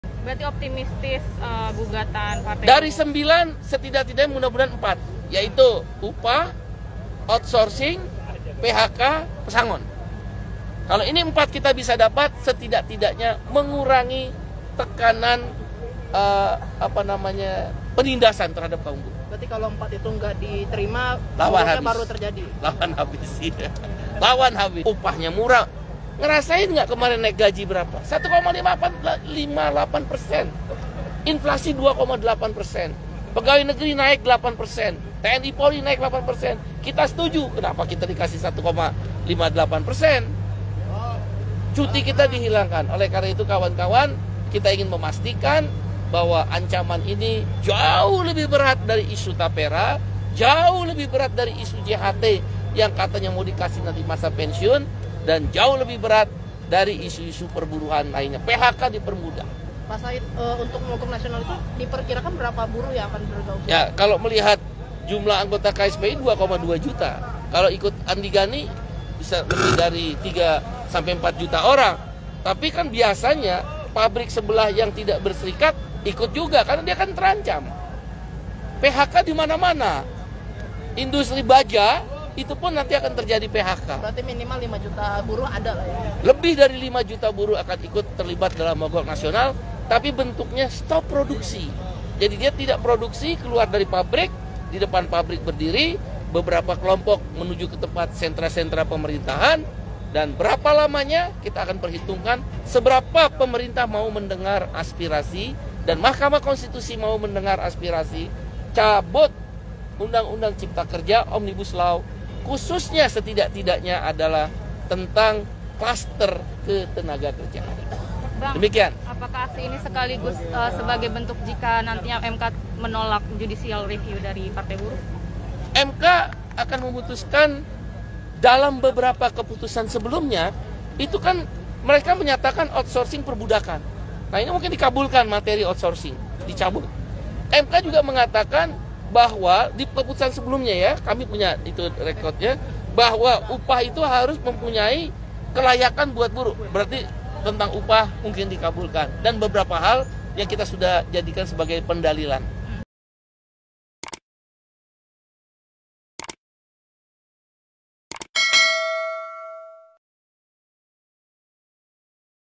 Partai Buruh Optimistis 4 Poin Judicial Review UU Cipta Kerja Dikabulkan MK
Presiden Partai Buruh Said Iqbal mengaku optimistis Mahkamah Konstitusi (MK) mengabulkan setidaknya empat poin judicial review Omnibus Law UU Cipta Kerja.
Dia menyampaikan hal ini saat massa buruh menggelar unjuk rasa di Patung Kuda Arjuna Wijaya, Gambir, Jakarta Pusat, Senin (8/7/2024).
Adapun buruh mengajukan sembilan poin dalam uji materi ke MK.
Said yakin, MK setidaknya bisa mengabulkan poin terkait upah, outsourcing, PHK, pesangon.
Menurut dia, apabila dikabulkan, empat poin itu bisa mengurangi tekanan dan penindasan terhadap kaum buruh.
Video Jurnalis: Xena Olivia
Penulis Naskah: Xena Olivia
Produser: Nursita Sari
Video Editor: Xena Olivia